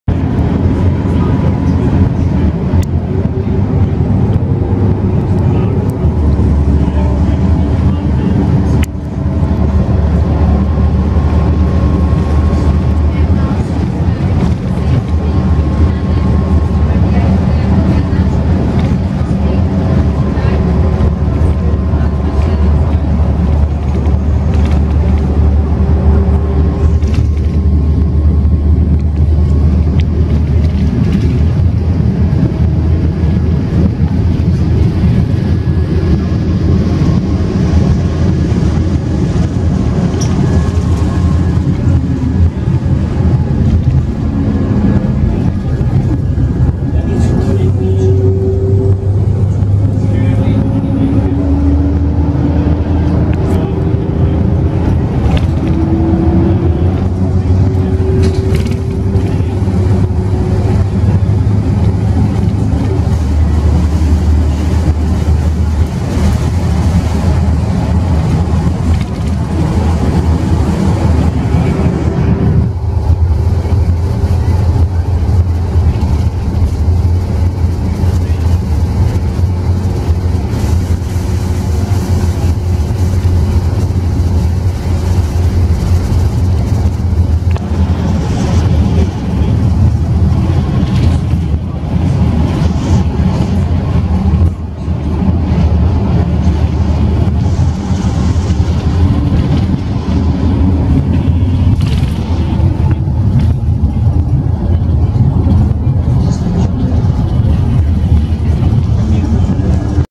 Arriva Midlands 3697 - YJ12 PLU - Shrewsbury
Filmed along the A458, between Shrewsbury & Bridgnorth, High Town.
Route 436
Optare Versa V1170 B43F

Route 436 passed to Easycoach (The Skiers Lodge) in September 2017.